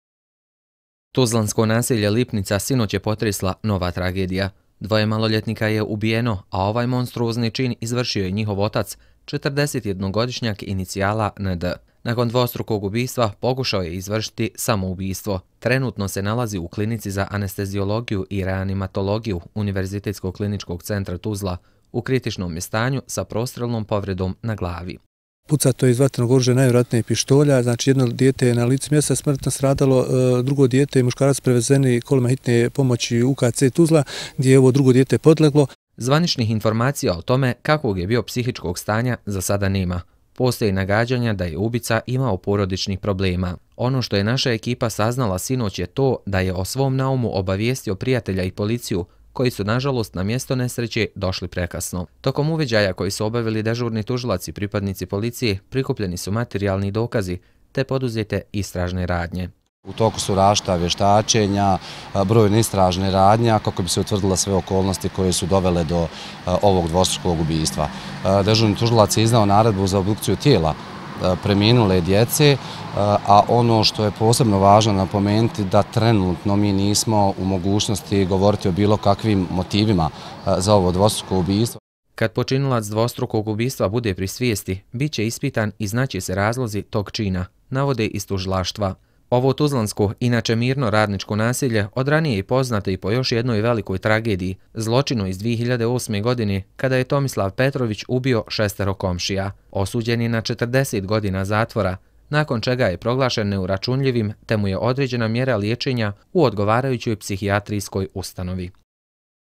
Otac ubio dvoje djece u Lipnici: Šta su motivi ovog čina? - 29.09.2016.
Dvoje maloljetnika ubijeno je sinoć u tuzlanskom naselju Lipnica. Ovaj monstruozni čin izvršio je njihov otac inicijala N.D. koji je nakon toga pokušao samoubistvo. Nalazi se u Univerzitetskom kliničkom centru Tuzla, gdje se ljekari bore za njegov život.